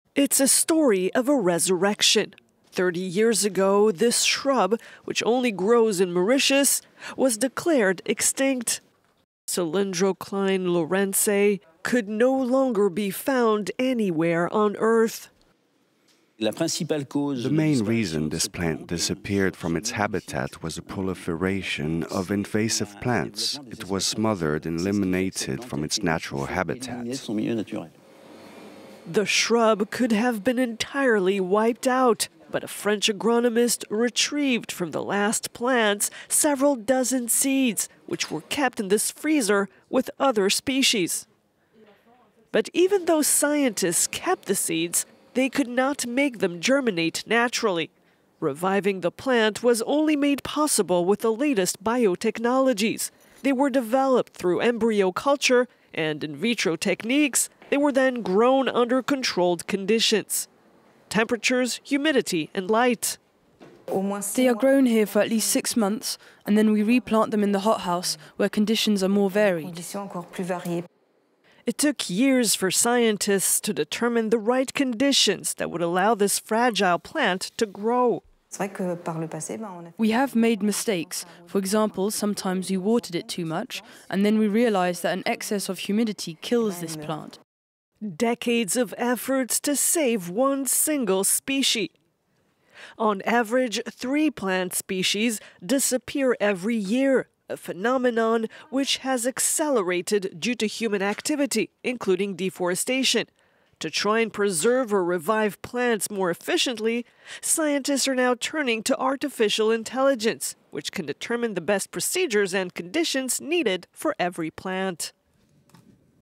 UN biodiversity summit : an indigenous plant saved and reintroduced in Mauritius • FRANCE 24
Monday 21 October sees the start of the biodiversity COP in Cali, Colombia. Two weeks of negotiations to guarantee the protection of animal and plant species. And here's a fine example of rebirth: a plant that has been extinct since the 1990s has finally been replanted in its original natural habitat. After years of research, botanists have succeeded in saving it.